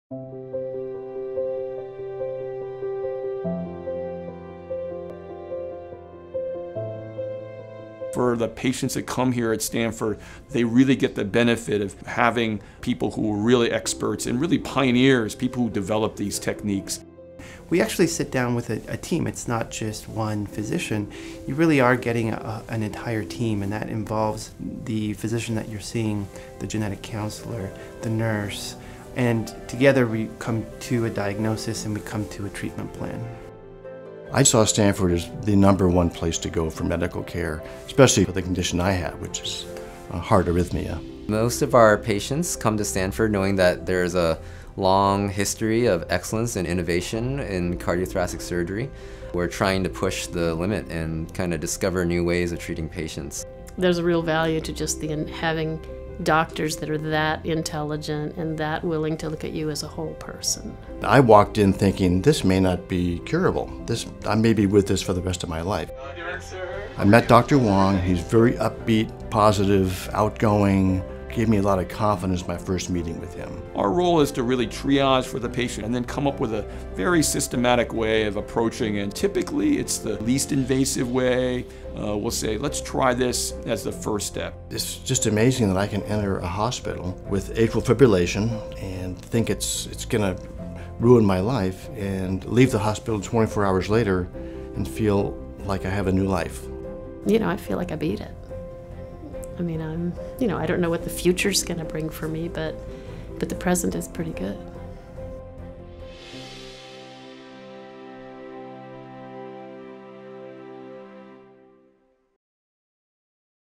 Stanford Health Care - Arrhythmia
Our doctors and patients discuss the leading-edge care each person receives at the Stanford Health Care’s Cardiac Arrhythmia Service.  Stanford has a long history of excellence in cardiac care, and our patients have access to that innovation through a team of specialists who work together to devise the best, whole-person treatment plan.